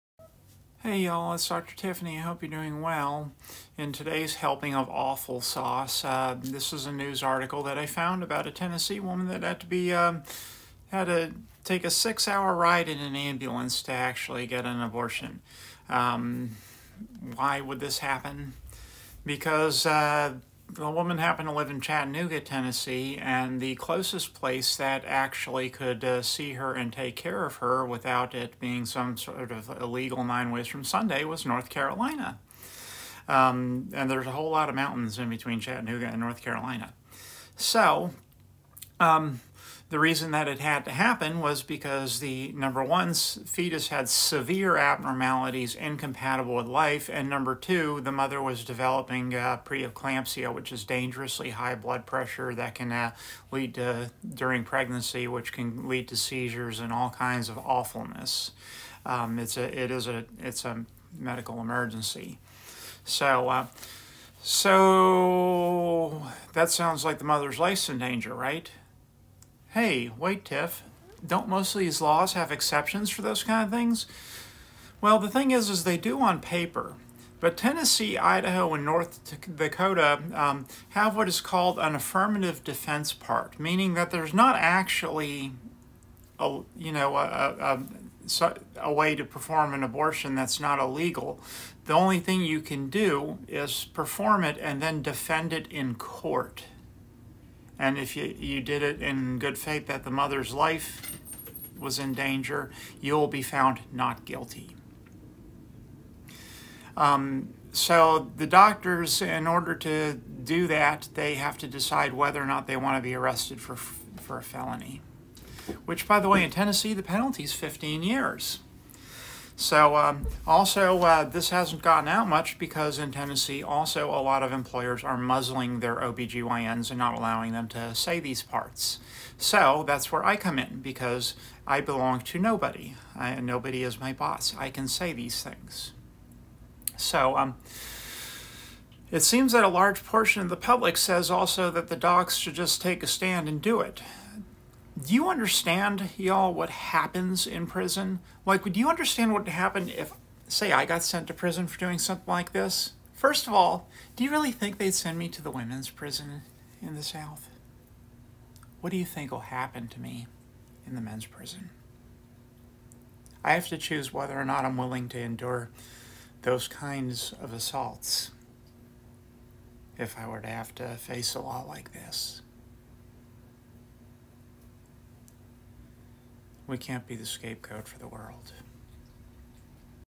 Guilty until proven innocent
In tennessee they charge doctors with a felony for performing abortions and it is up to the doctor to prove it was medically necessary to save the mother’s life. So if you are a doc, make sure you have vail money at all times….